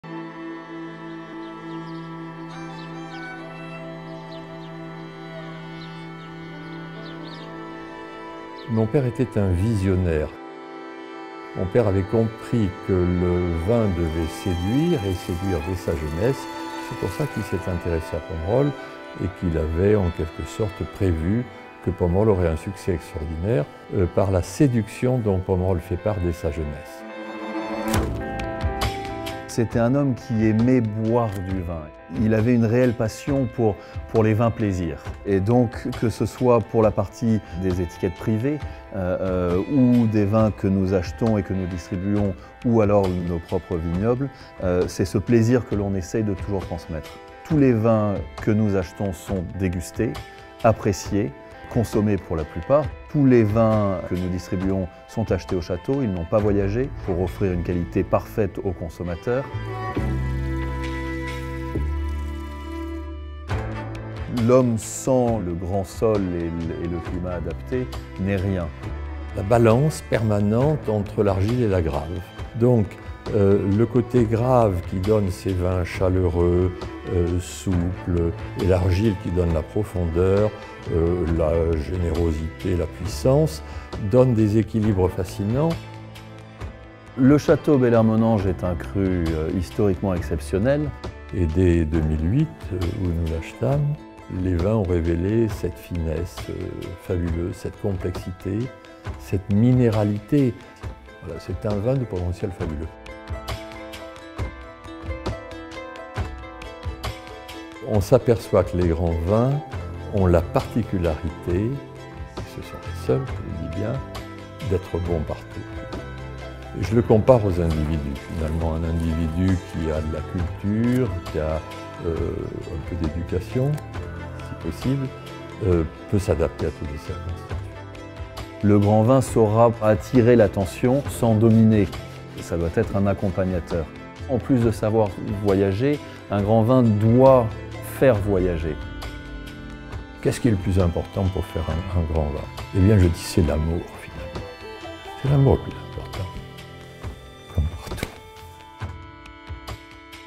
Defining a Great Wine : Interview with Christian and Edouard Moueix (English subtitles)
English subtitles: Christian et Edouard Moueix present Jean-Pierre Moueix Grands Vins and the principal family vineyards.